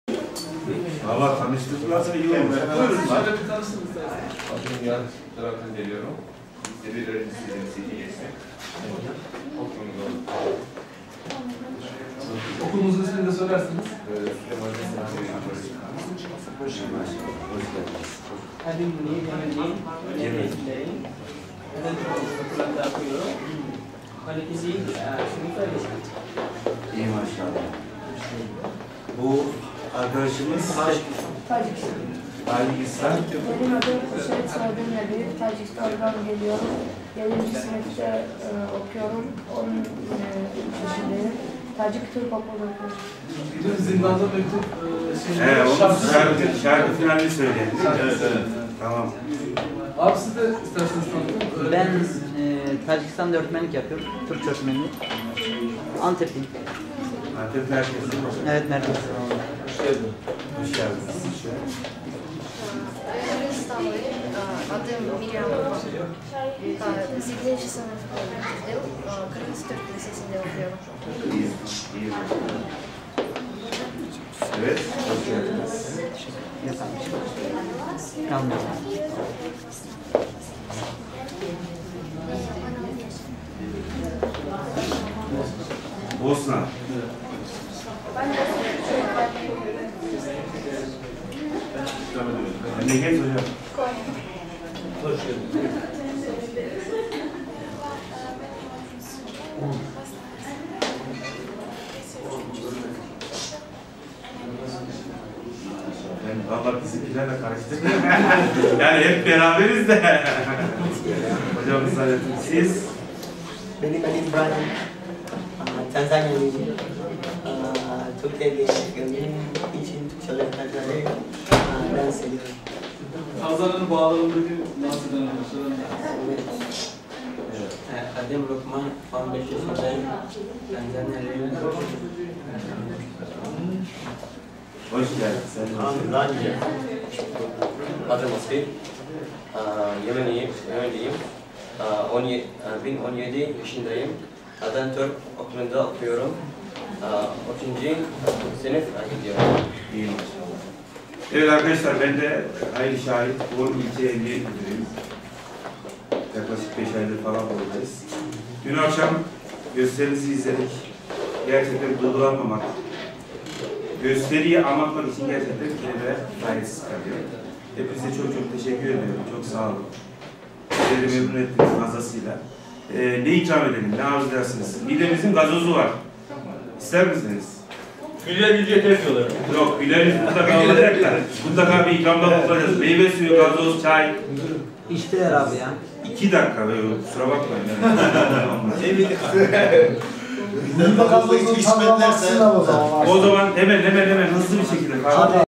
Olimpiyat Çocukları Bor İlçe Emniyet Ziyareti (AÇI NET HABER)
Olimpiyat Çocukları Bor İlçe Emniyet Müdürü Hayri ŞAHİN'i  Ziyaret Etti